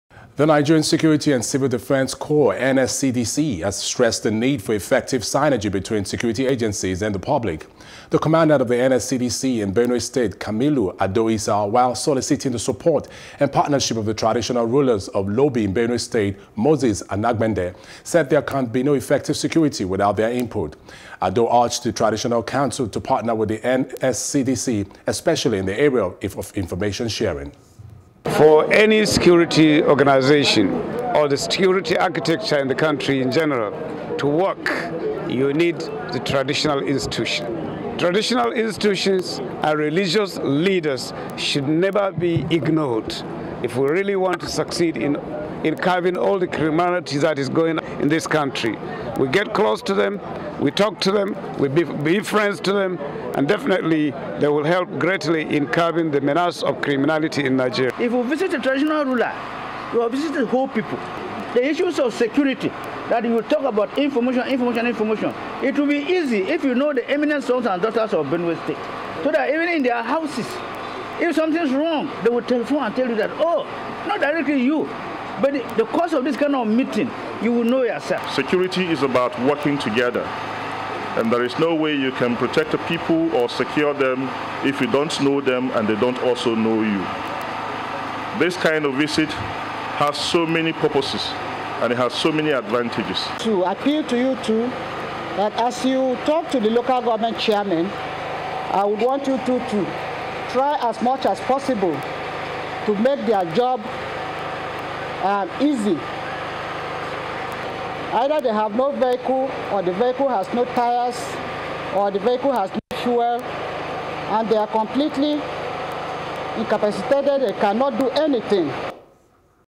NSCDC Seeks Partnership With Public, Traditional Council In Benue
The Nigerian Security and Civil Defence Corps NSCDC has stressed the need for effective synergy between security agencies and the Public